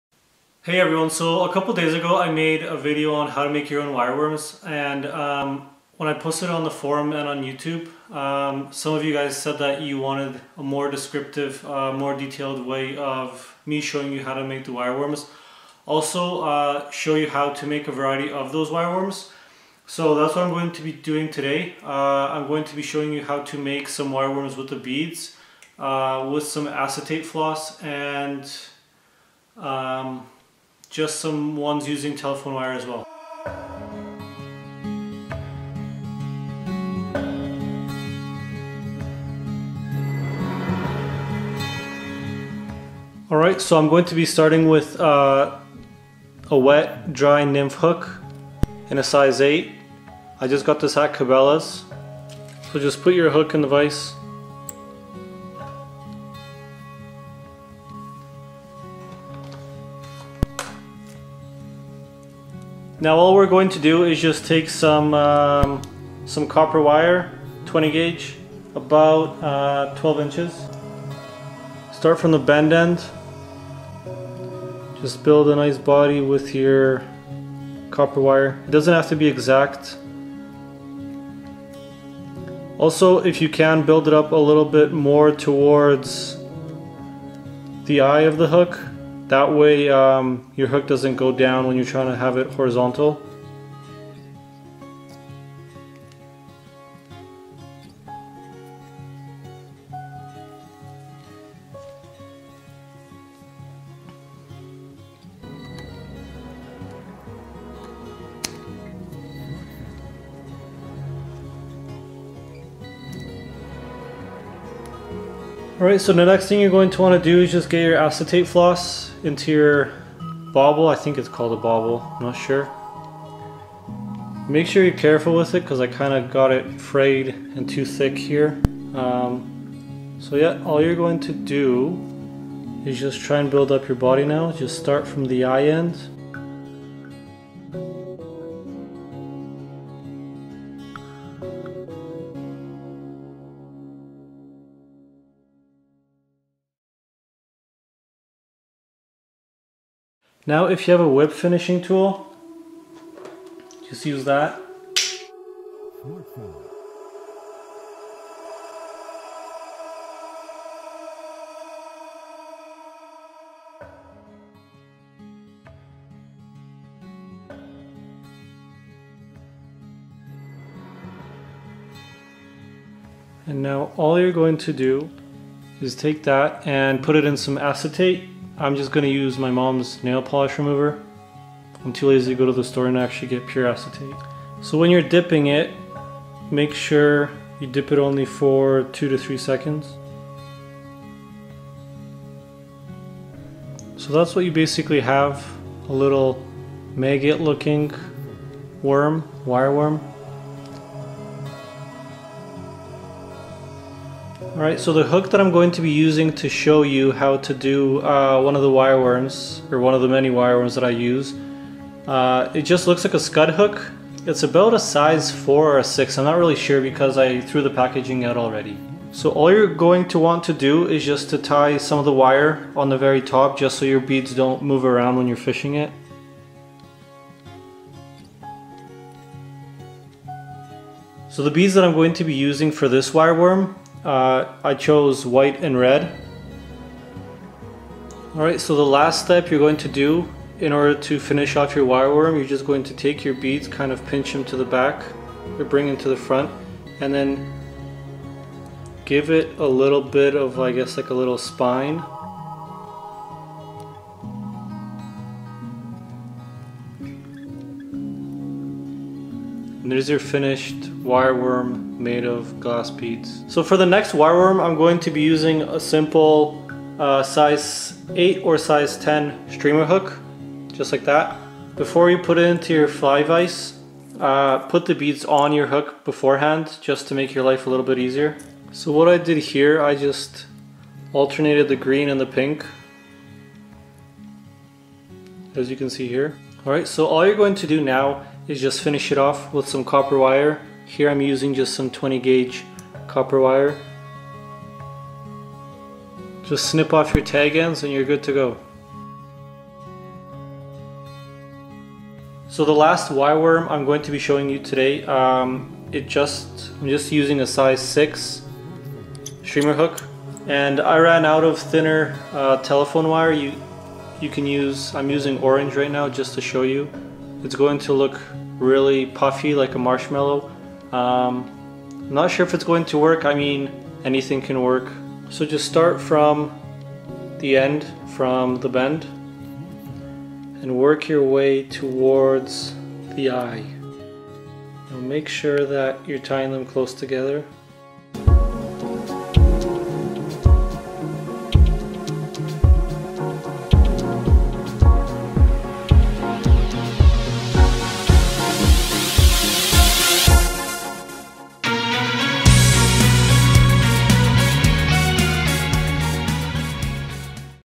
How to make wire worms- Part 2
So after making a video on how to make wire worms for ice fishing, or any type of fishing for that matter; I received some helpful feedback. I was asked to go a little bit more into detail about how to make different types of wire worms and what hooks to use etc.

Materials List: -any color of wire (14-20 gauge) -fly tying vise grips -streamer hooks, or hooks of your choice -nail clippers or wire cutters (to cut the wire easily) -colored beads -different colours of fly tying threads -imagination